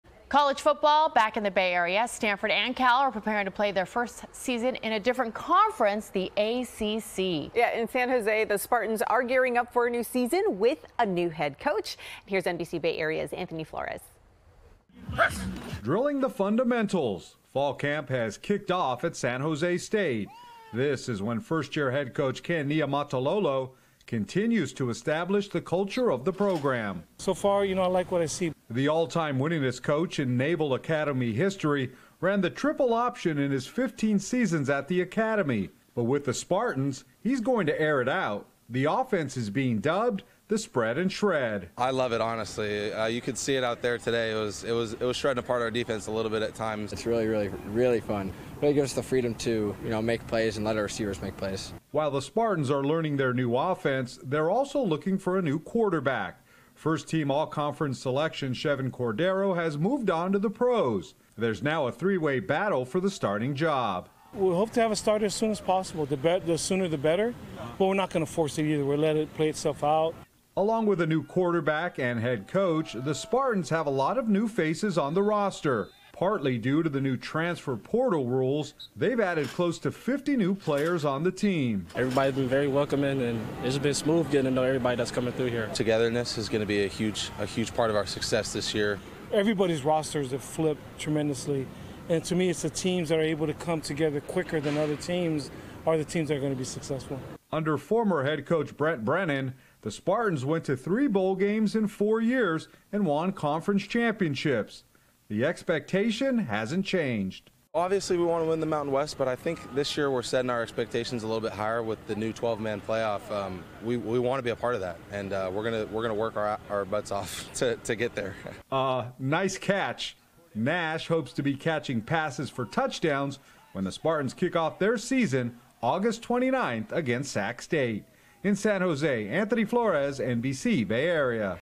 SJSU football preview
San Jose State University's football program is coming into the season with a new coach as the program looks to rebuild. Anthony Flores reports.